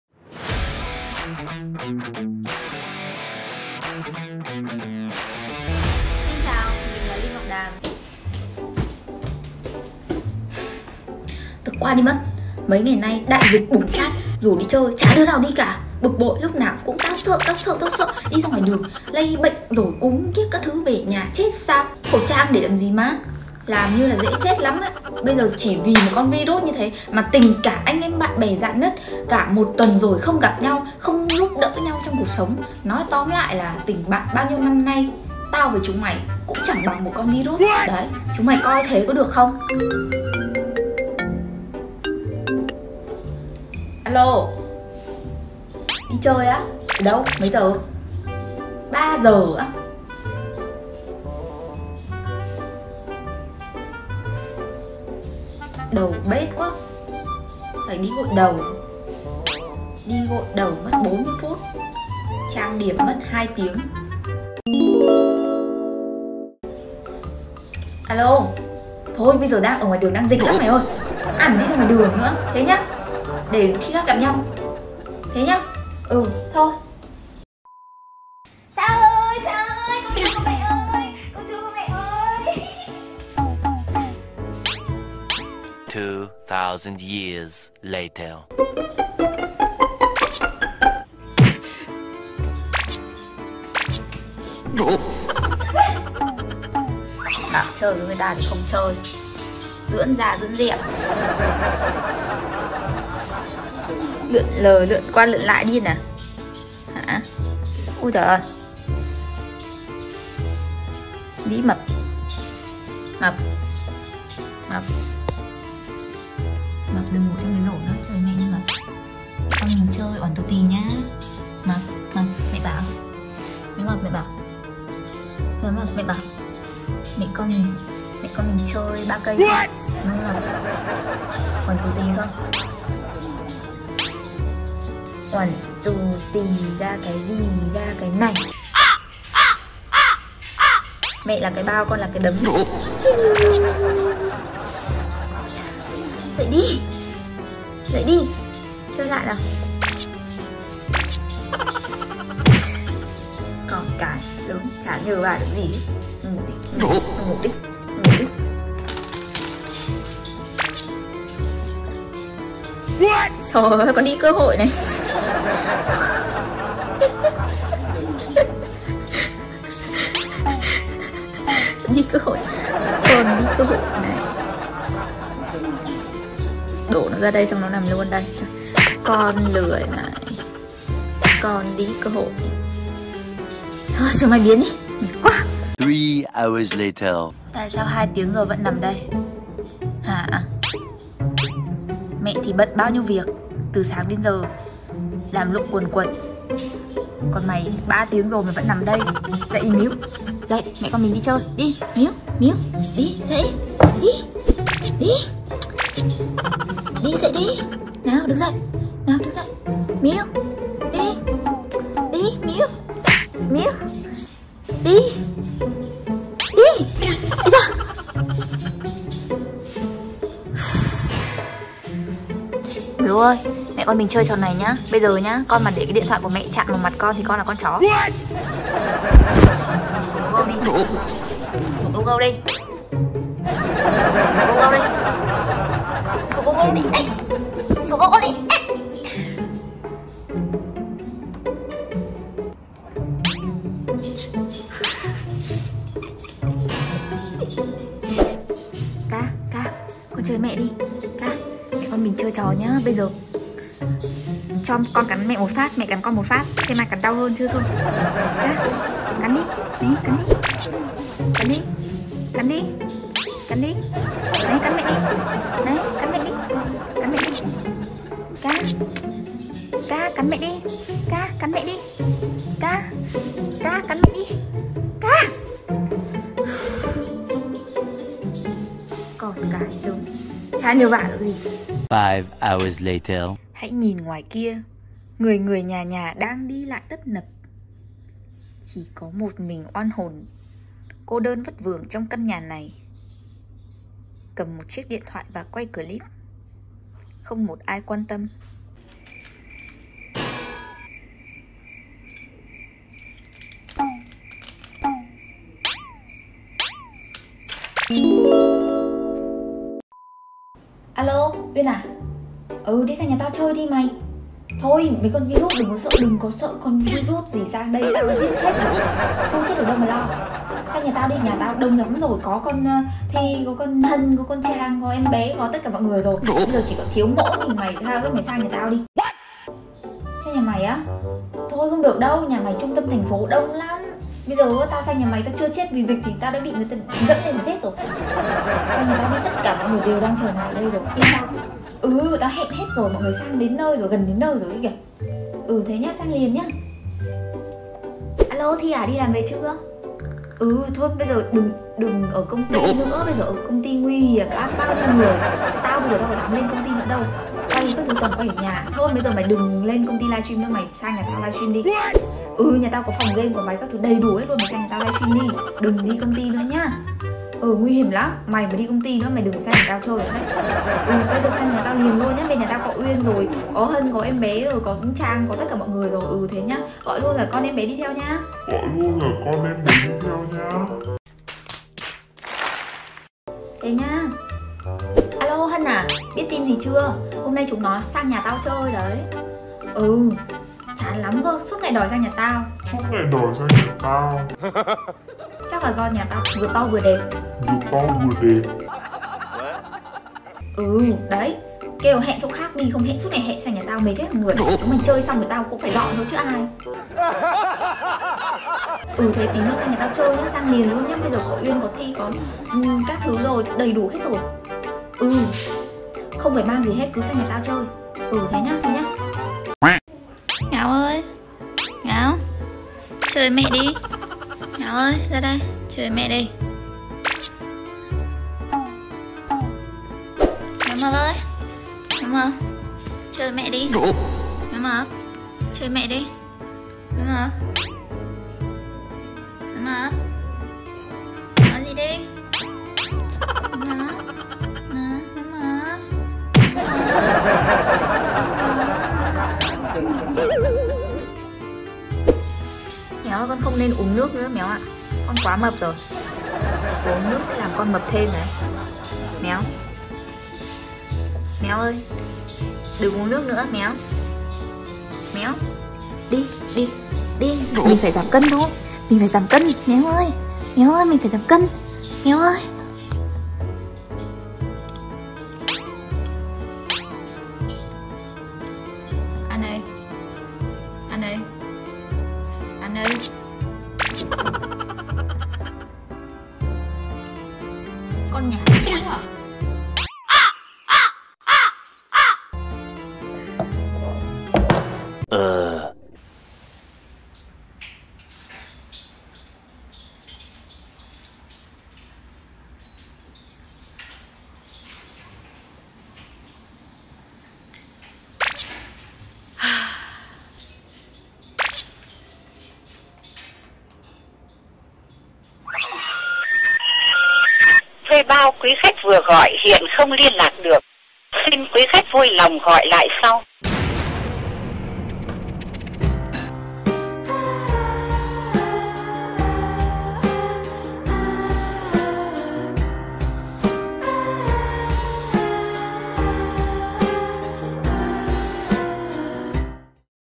HẸN HÒ MÙA DỊCH VÀ CÁI KẾT | LINH NGỌC ĐÀM
Cô-rô-na làm Linh cô đơn quá mọi người ạ. Không biết Valentine này sẽ thế nào đây? hay là lại phải chơi cùng Míu và Méo nữa đây 😥

CREATORY PRODUCTION
© Bản quyền thuộc về Linh Ngọc Đàm